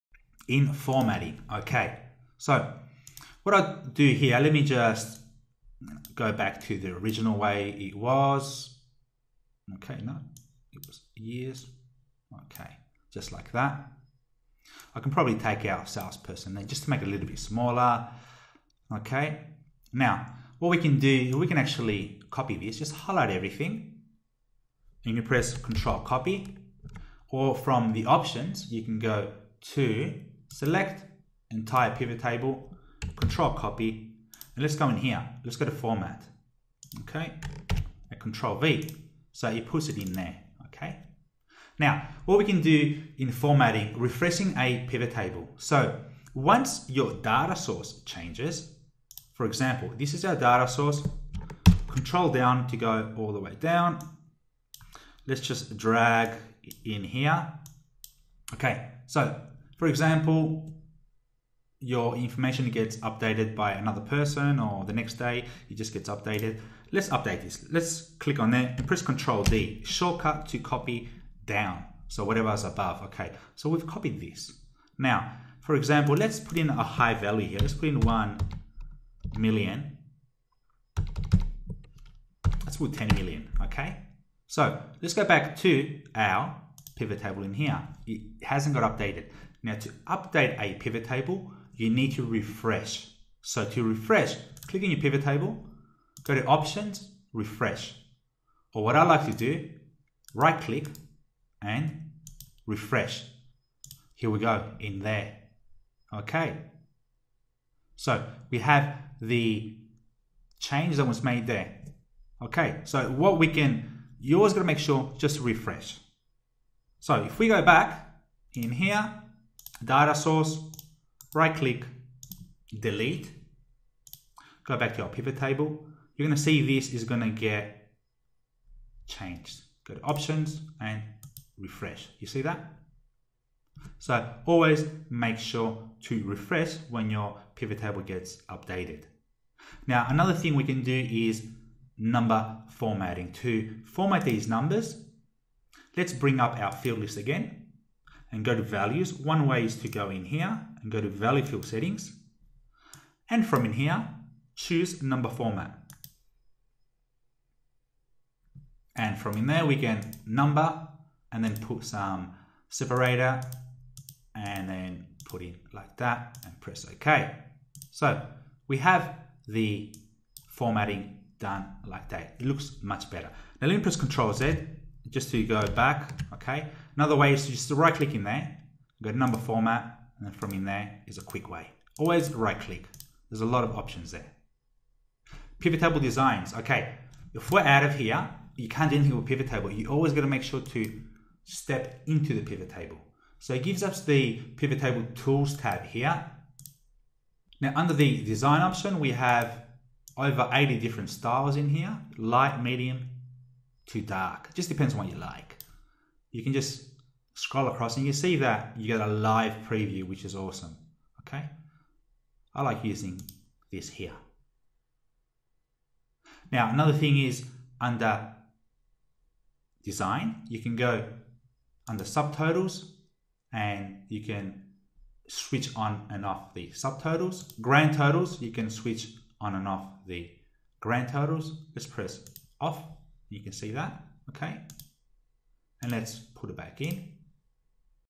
Formatting an Excel Pivot Table
When you create a pivot table, a default PivotTable Style is automatically applied. You can change the default PivotTable Style, if you prefer a different format.